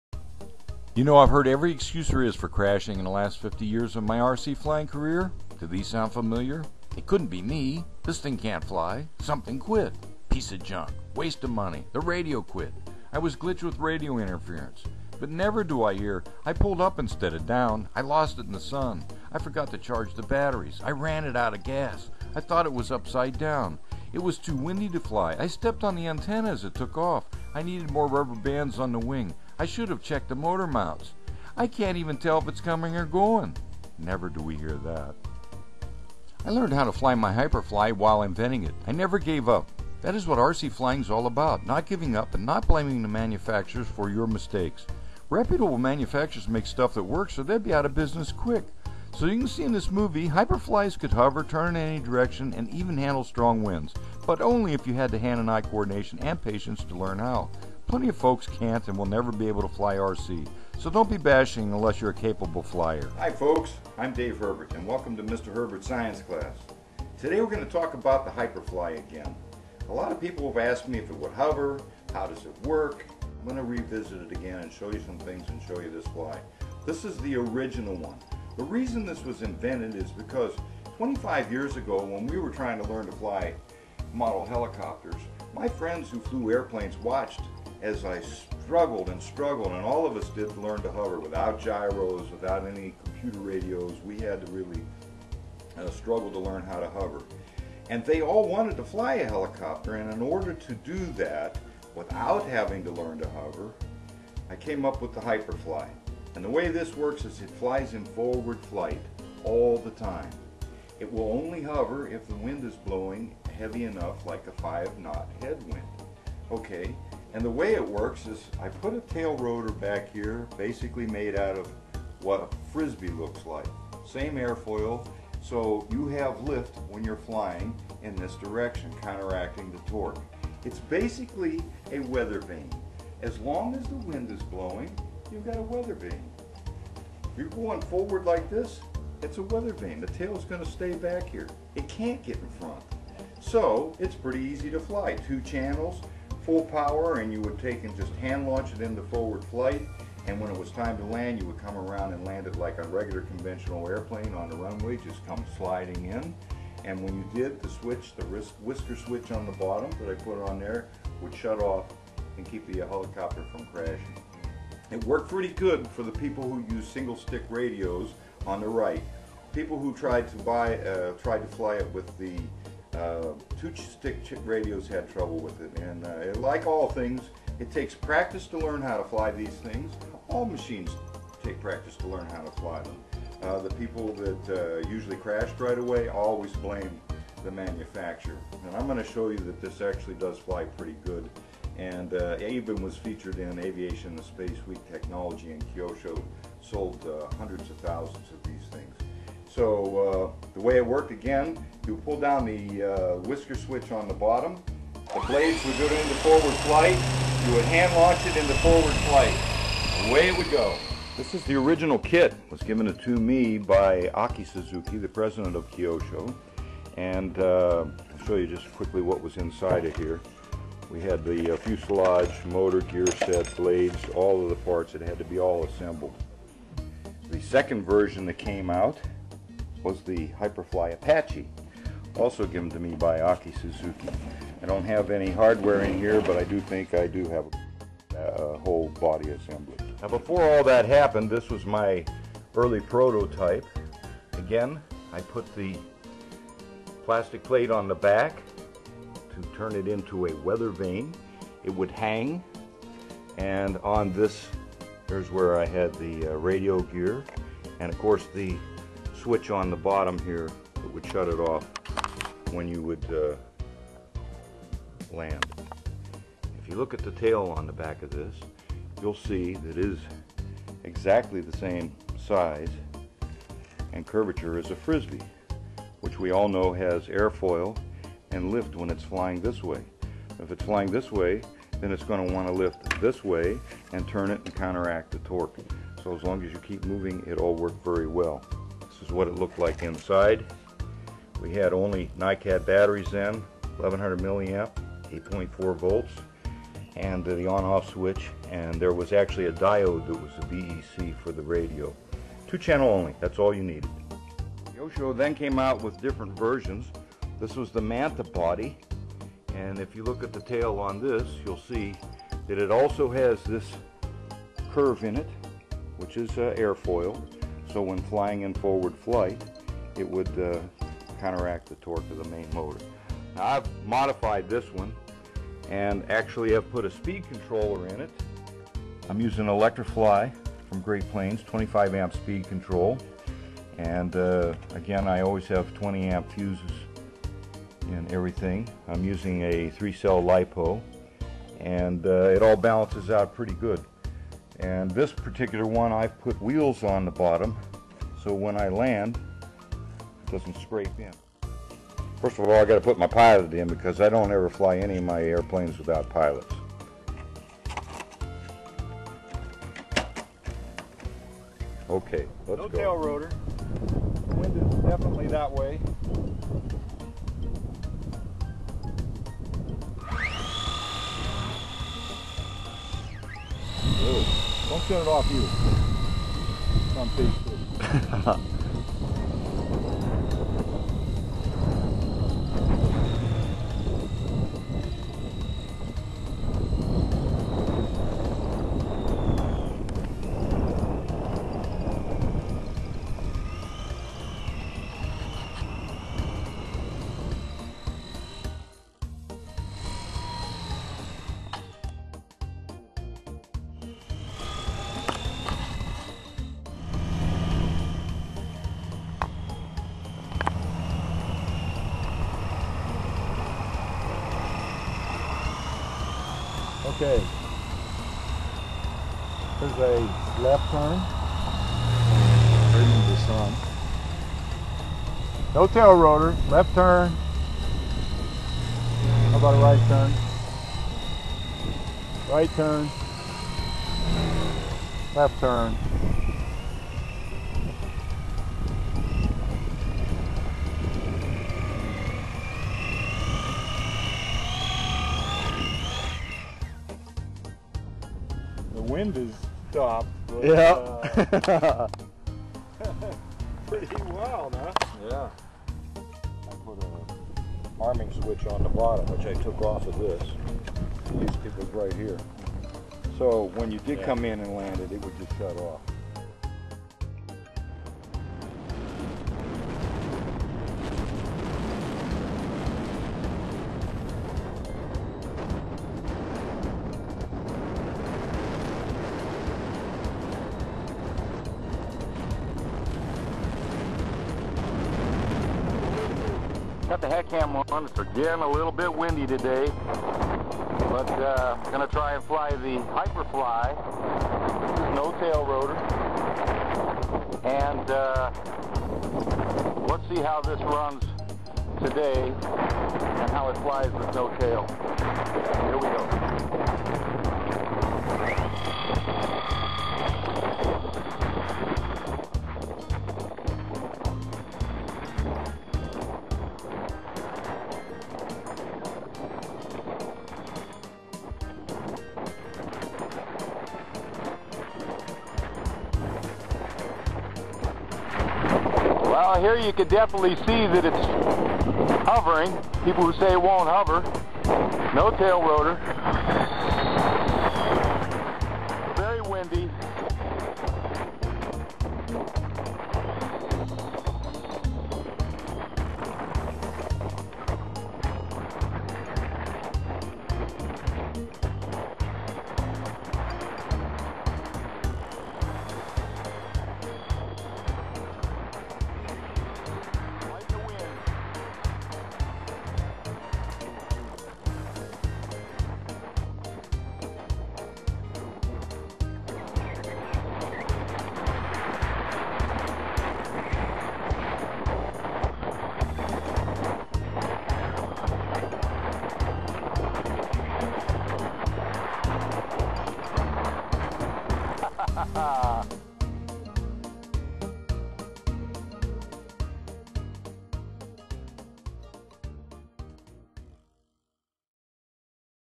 Will the Kyosho Hyperfly NOTAR Hover?
People say manufacturers stole my idea for ETRMS and this NOTAR.

Listen, It was never stolen!  I simply was messing with inventing and doing untried stuff back when RC was new.

I never tried to sell the idea, just show them off, so I sent articles and pictures and vids to the magazines and manufacturers.
 I  just wanted to show what I could do and they could print it, which they did.

I know all the people who used the ideas, especially Aki Suzuki (owner of Kyosho) who paid me very well for 8 years for this Hyperfly Idea.

Many people said it could not hover or even fly at all for that matter.

Mode I Radios made it tough for USA pilots who can't fly airplanes in Mode I either.

Once Great Planes began selling them with a radio, I tried to not allow them to  sell it with a Mode I radio,but it was out of my hands by then and Futaba had a ton of left over Mode 1 radios, and made the deal.  I was disappointed in that.

But it really sold well overseas, where they fly mode I.

Because this machine was new, and unconventional, it was beyond most Mode II fliers flying capability who had no  understanding of basic helicopter theory and aerodynamics.

Here I show exactly what it could do. (And of course in the right hands), they were proven wrong, as I demonstrate the extremes in this video.

(some people can't drive you know, and if you dare to learn something new, with practice, you can master anything, as many of us could really fly the Hyperfly great.  But most not, and it is still the same today)
Helis are difficult.

The Hyperfly turned out to be the most sold model of all time in Kyosho's history.  Most of the sales came from overseas where people did not give up learning to fly it like they did in the USA.
It ran it's sales course for 8 years, which is longer than most innovative flying machines that typically have a sales life expectancy of only 2 years or less.

It was featured in Aviation Weekly and Space Technology magazines. The original is on display at the San Diego Aerospace Museum which was donated by Aki Suzuki of Kyosho Corporation.

I had to successfully fly it for the museum staff first, to prove it's airworthiness.

This machine was originally designed by me to prove a point. And that point was that I could take any electric ETRM helicopter, take the tail rotor motor off, put a plastic plate on the tail and fly around just fine.

You may have seen it in one of my other Hyperfly videos on youtube.

It was a new kind of flying machine at the time, that only needed a 2 channel radio to fly.

I hope you enjoy this bit of more R/C history folks.

Happy flying.